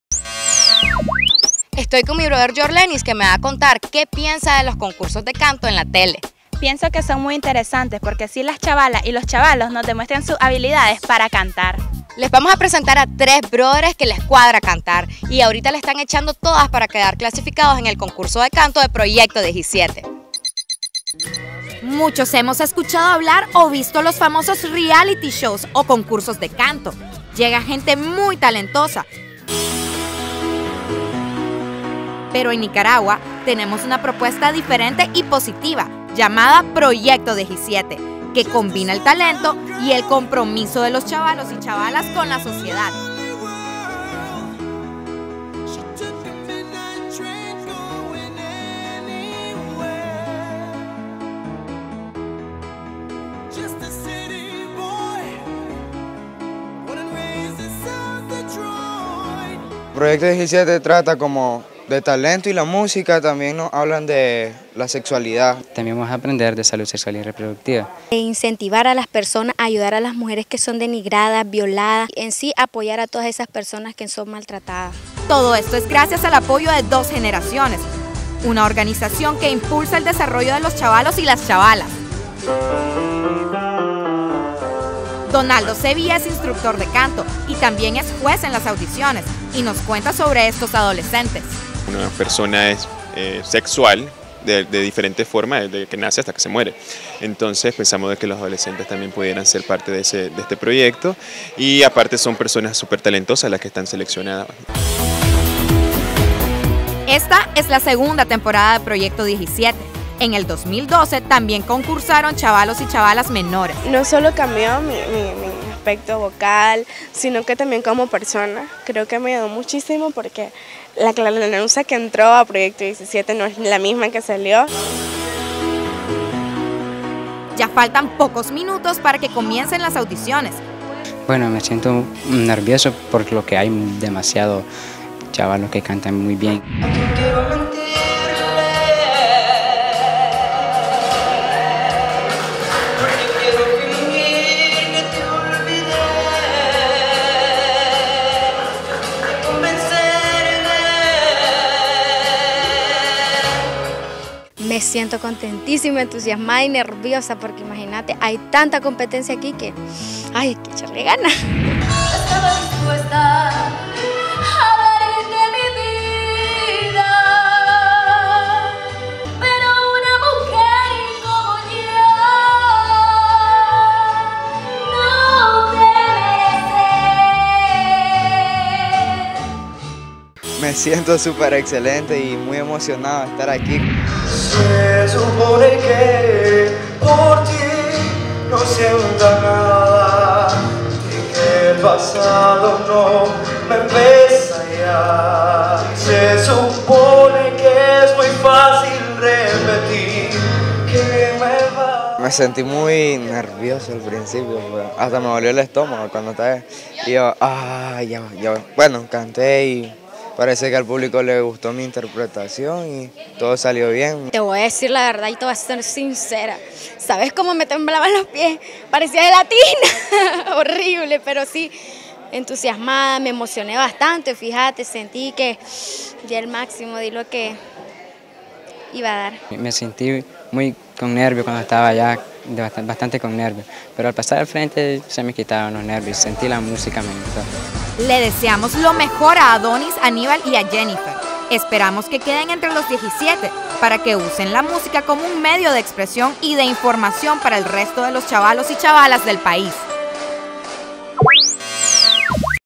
Proyecto 17
Te presentamos a Proyecto 17 un concurso de canto que pretende formar a los jóvenes no sólo música sino que también animarlos a hablar sobre los problemas de su entorno.